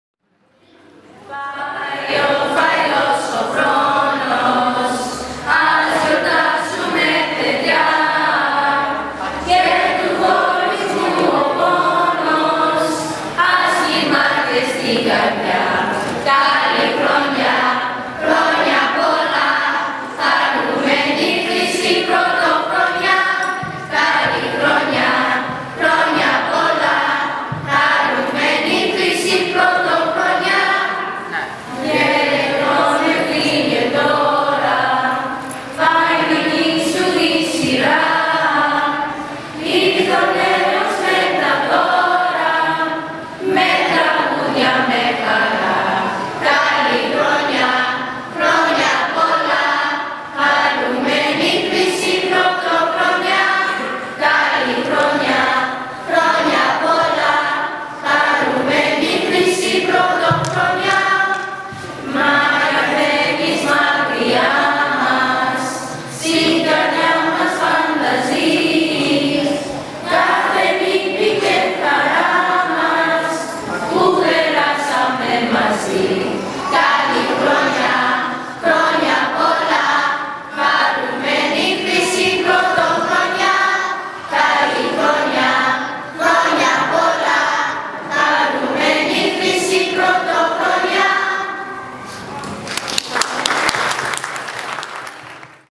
2014 New Year Greeting from the Greek School students of Evangelismos
The Greek School students of Evangelismos Greek Orthodox Church sang out a New Years greetings to family members friends and beaming parishioners who looked on last week.

The afternoon event, included poems and holidays songs.